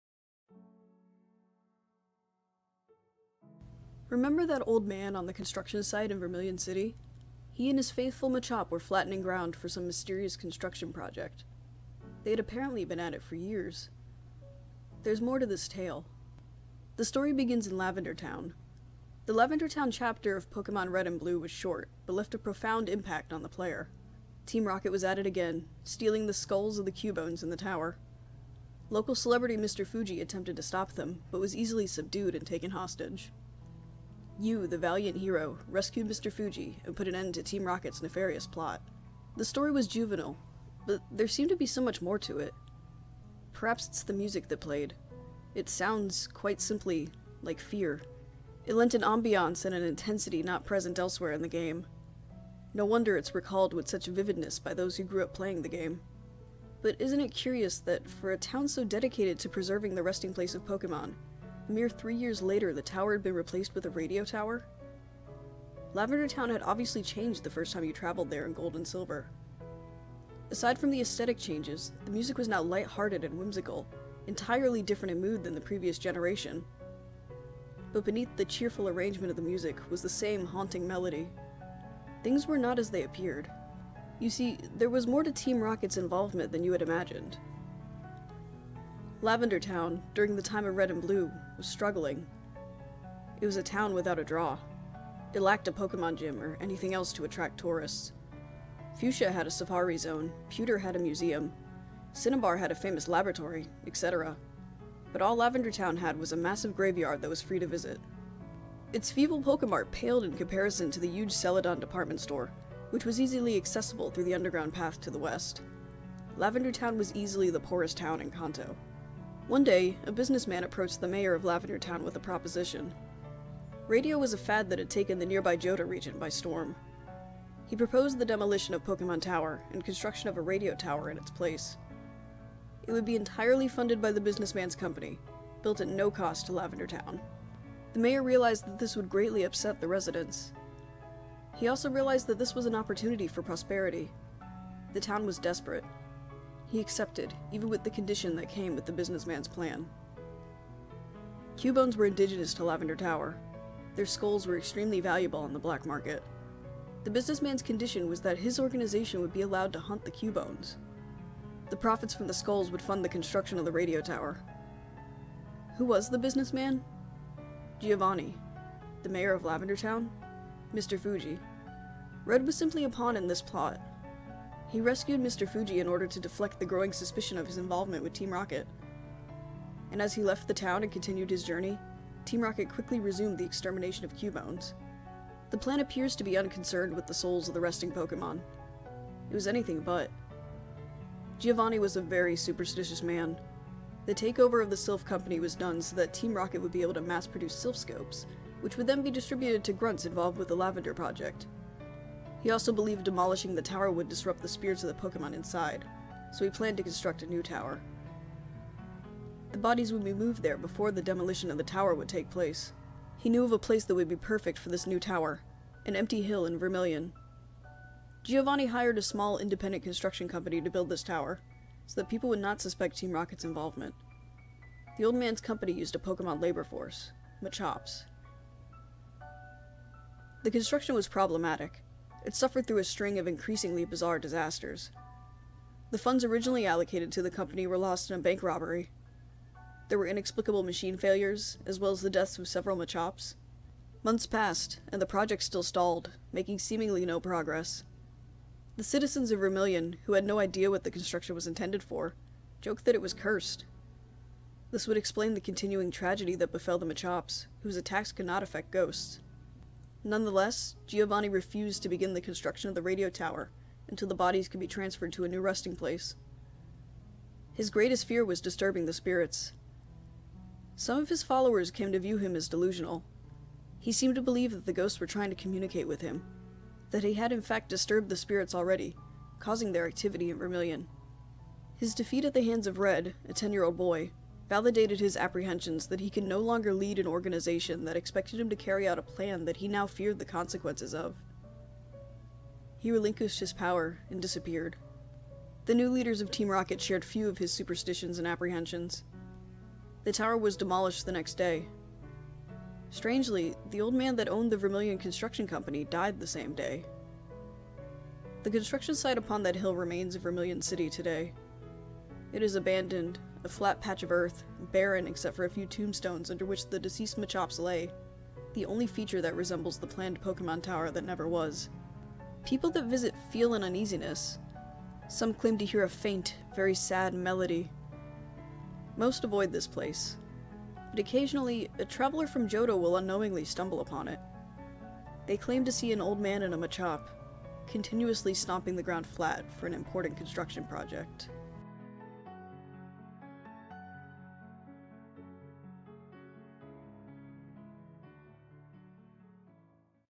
Vermilion City Construction Project | Pokemon Creepypasta
I know basically everyone's done this one, but I wanted to start off this month with one of my favorite PokePastas.